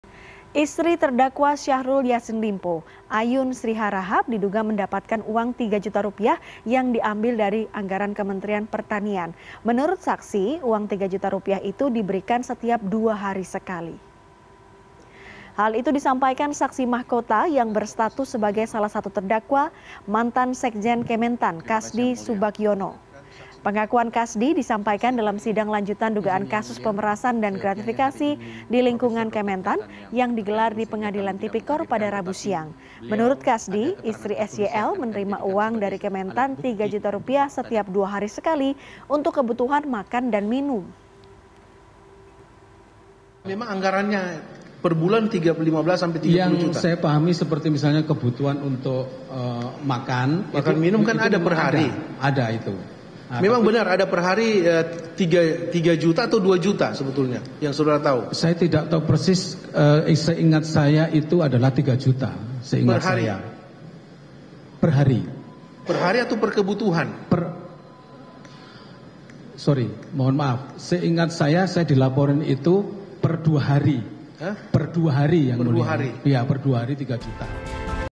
Saksi: Istri SYL Dapat Rp3 Juta Per 2 Hari dari Kementan
Istri terdakwa Syahrul Yasin Limpo, Ayun Sri Harahap, diduga mendapatkan uang 3 juta rupiah yang diambil dari anggaran Kementerian Pertanian. Menurut saksi, uang 3 juta rupiah itu diberikan setiap dua hari sekali.